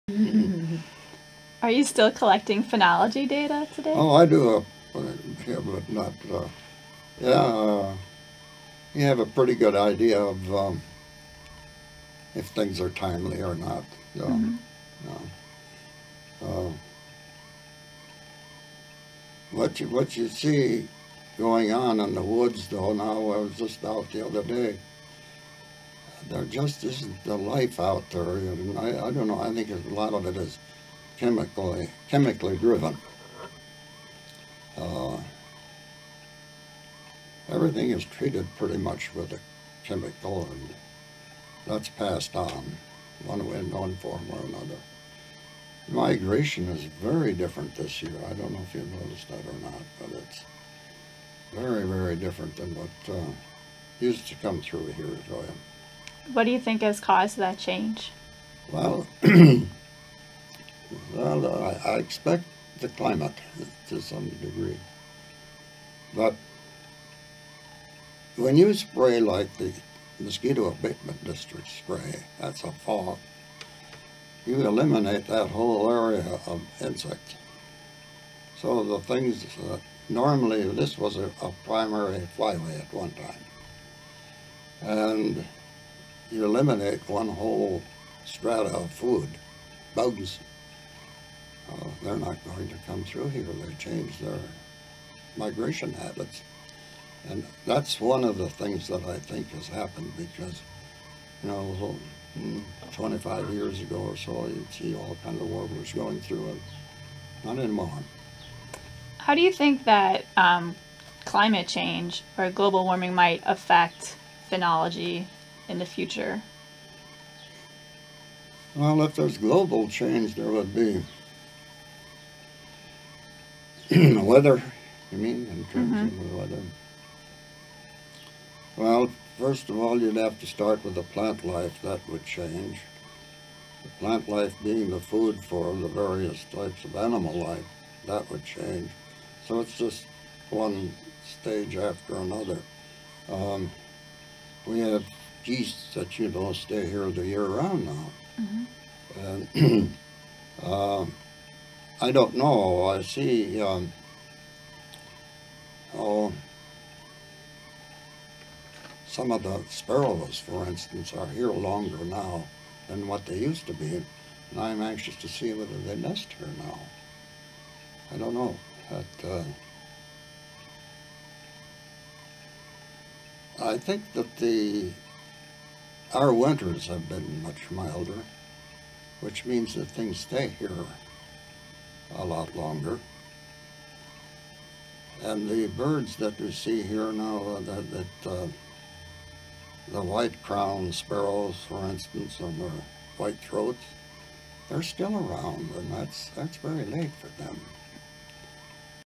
Roland Eisenbeis Interview (excerpt 2)
Interview with Roland Eisenbeis, Superintendent of Conservation for the Cook County Forest Preserve from 1952 to 1984.

Interviewers: Kelly Ksiazek and Rebecca Barak